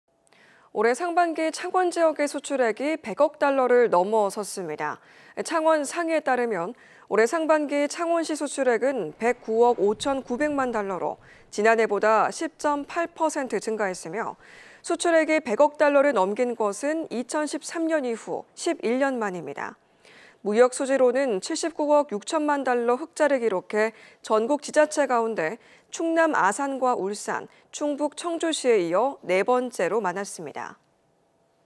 창원 상반기 수출액, 100억 달러 넘어 / KNN
올해 상반기 창원지역의 수출액이 100억 달러를 넘어섰습니다.

 / knn.news